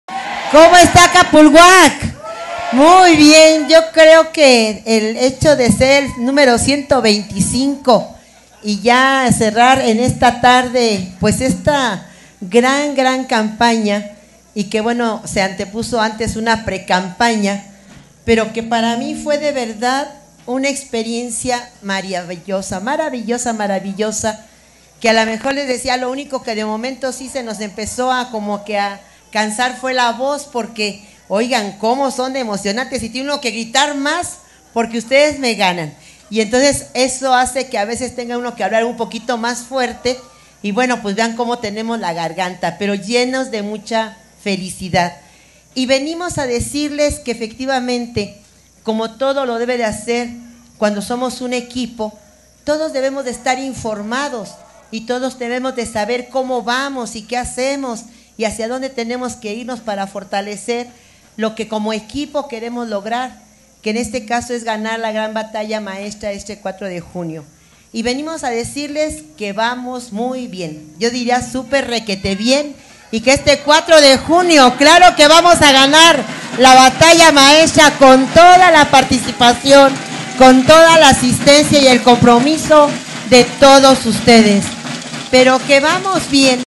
Delfina Gómez Álvarez, cierra en Calpuhuac
"¡SOY MUJER DE TERRITORIO!"; VISITA DELFINA GÓMEZ LOS 125 MUNICIPIOS DEL EDOMEX

• En 58 días de campaña, la candidata a gobernadora se reunió con más de un millón de personas

CAPULHUAC, Estado de México.- Demostrando que la mejor forma de escuchar las necesidades de la gente es recorriendo el territorio, este martes, la maestra Delfina Gómez, candidata a gobernadora del Estado de México por MORENA, PT y PVEM, logró su meta de visitar los 125 municipios mexiquenses rumbo a la elección de este domingo 4 de junio.

Desde el primer día en el Teatro Morelos de Toluca, hasta hoy en el municipio de Capulhuac, la maestra Delfina Gómez ha cumplido 58 días de campaña recorriendo carreteras, caminando calles, visitando mercados, y recogiendo peticiones y propuestas para nutrir su plan de gobierno.

“Visitar los 125 municipios no fue para tomarse la foto, sino para escuchar y recibir propuestas, peticiones y preocupaciones, para así conocer de primera mano cómo está la economía de las familias mexiquenses, la seguridad, el abastecimiento de agua, la condición de los caminos, la situación del agua, la seguridad, las escuelas, y sólo recorriendo el territorio lo podemos saber”, expresó la maestra Delfina Gómez.

Más de un millón de mexiquenses asistieron a alrededor de 130 mítines que la maestra Delfina Gómez para escuchar sus propuestas de campaña.